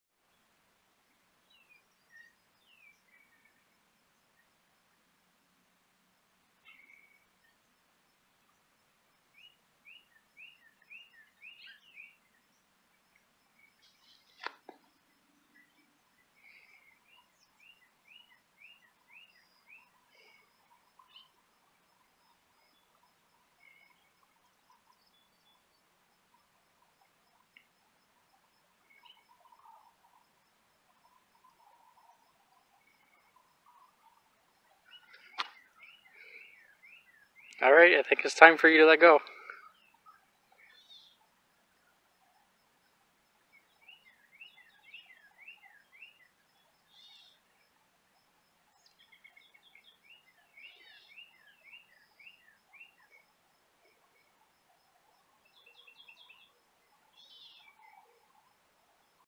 The Garter That Didn't Let Go
I picked up an Eastern Garter that was on the crawl and it held onto my hand tightly and didn't let go for some time after I placed it on the ground. About a minute after the video was taken, it finally loosened its grip and slithered off.

This is the first time I had a snake do this. strange but funny.